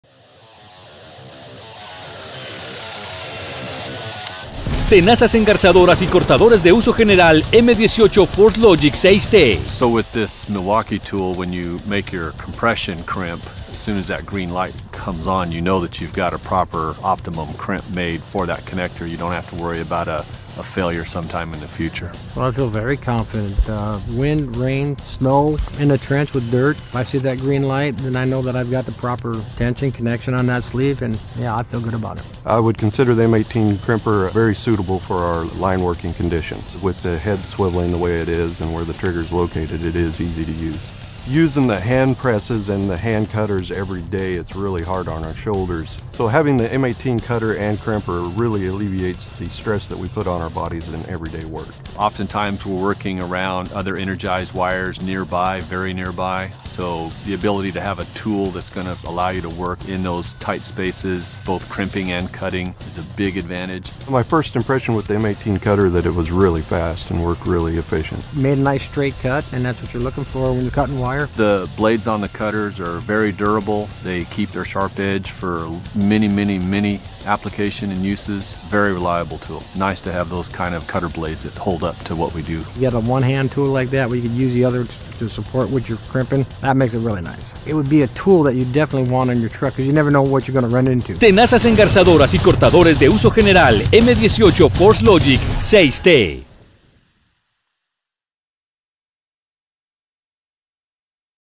Crimpeadora y cortadora Milwaukee® M18 ForceLogic™ de 6Toneladas
FORCE LOGIC™  está diseñado no solo para mejorar, sino para cambiar la manera en la que se usan las herramientas hidráulicas en el campo. Al mover el gatillo y agarre al centro de la herramienta, proporciona el mejor control con guantes de clase 3, mejorando la alineación y confiabilidad de la misma.